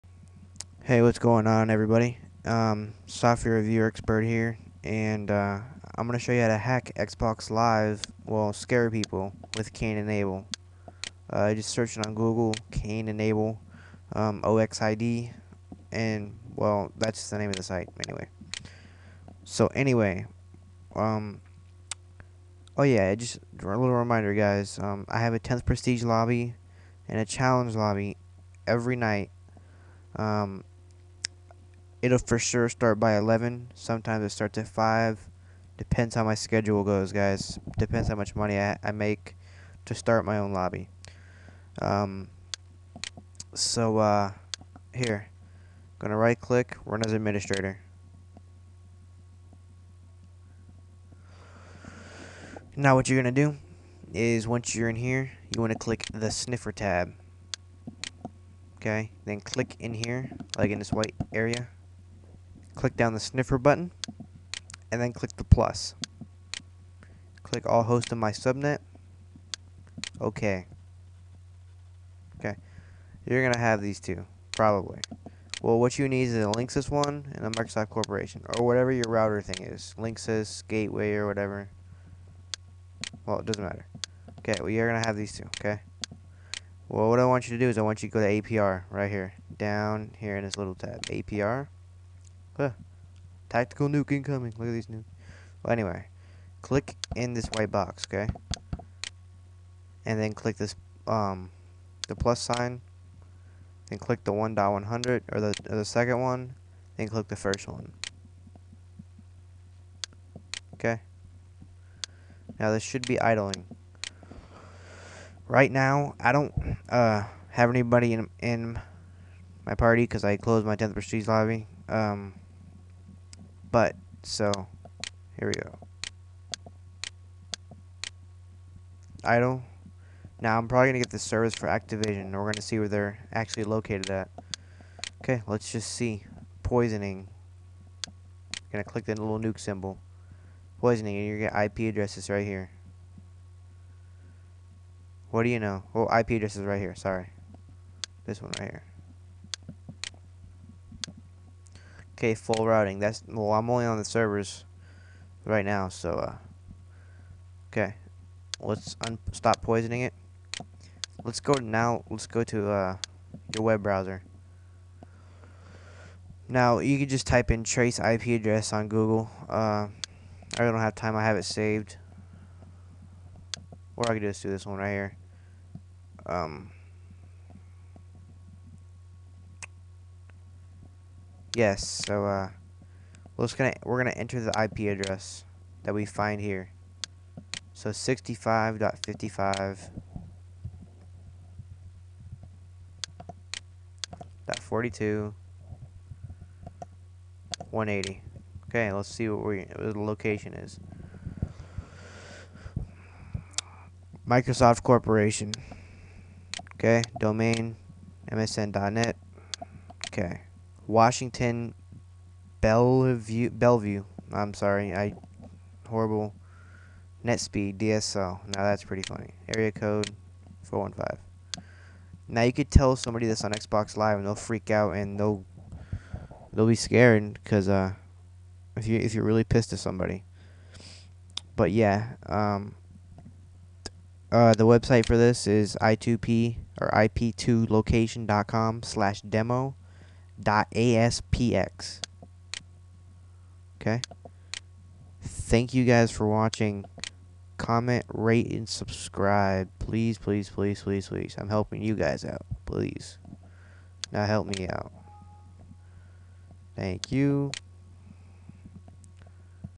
Find Xbox Live IP Addresses
Just for fun i found Microsoft's ip and traced them down